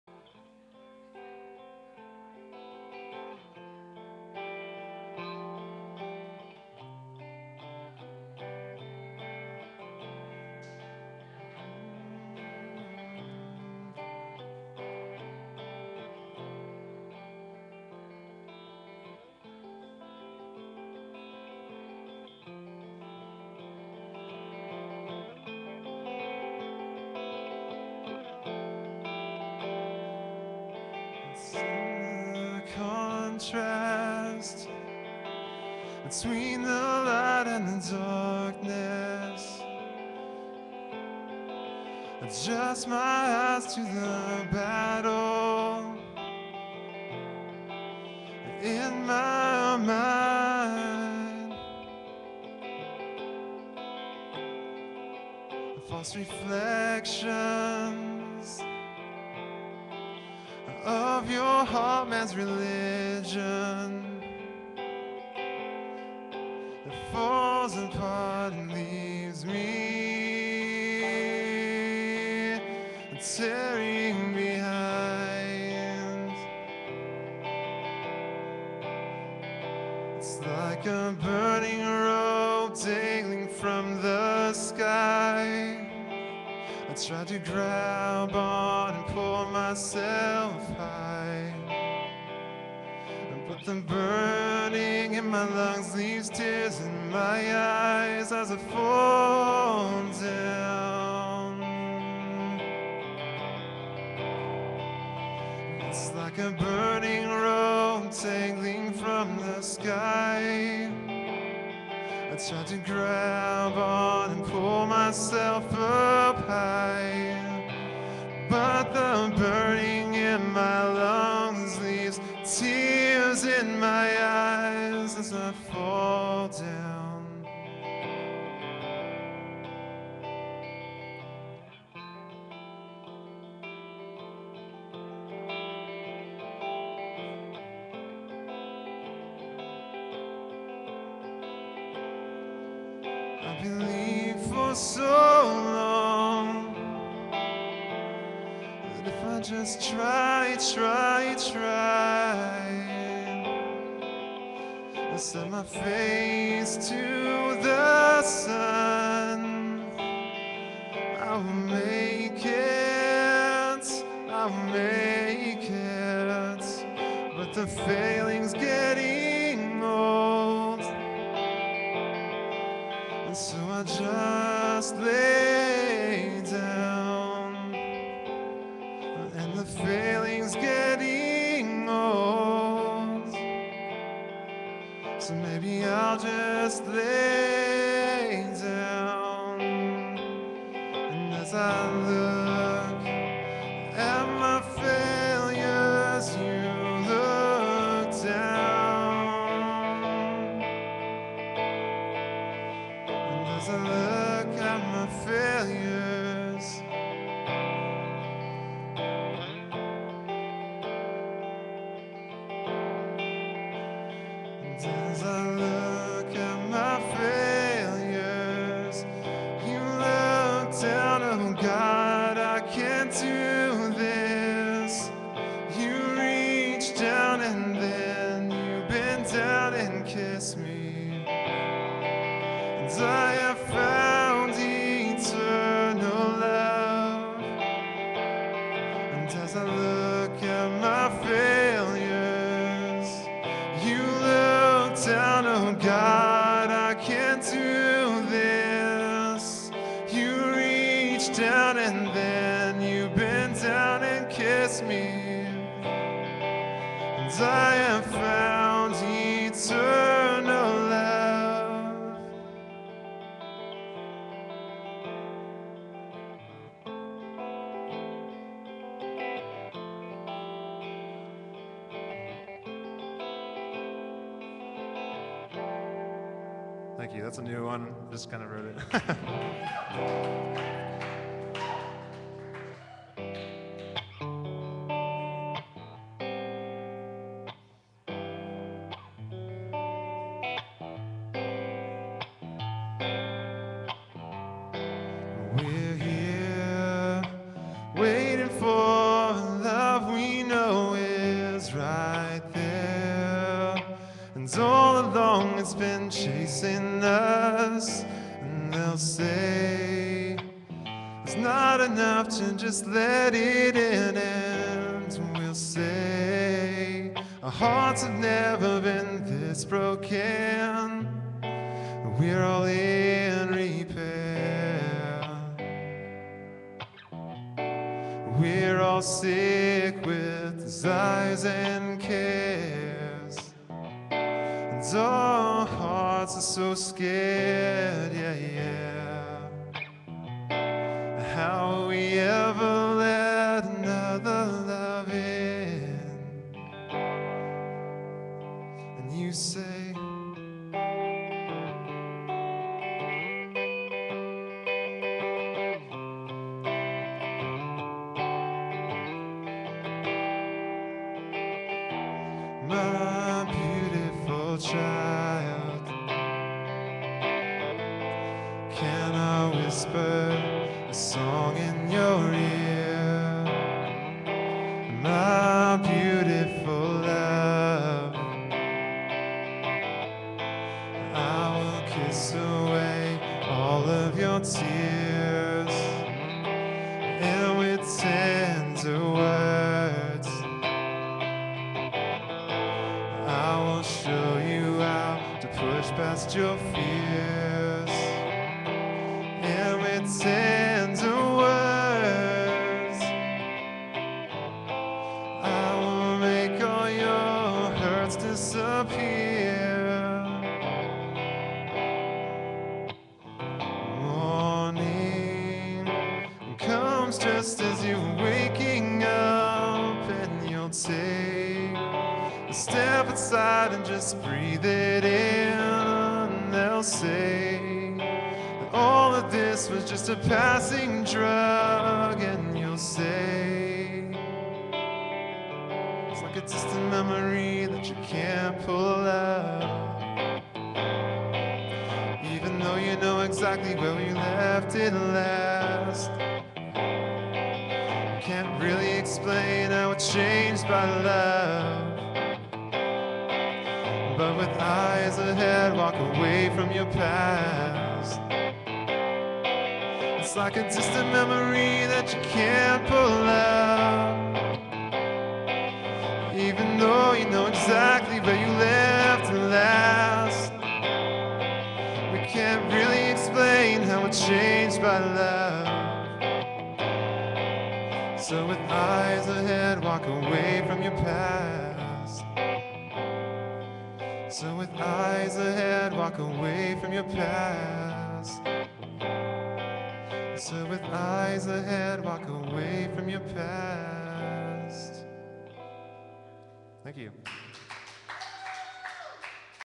CafeLift's Shaun - August 26, 2009
Shaun performs at CafeLift. CafeLift is an open mic cafe in San Jose CA. Check us out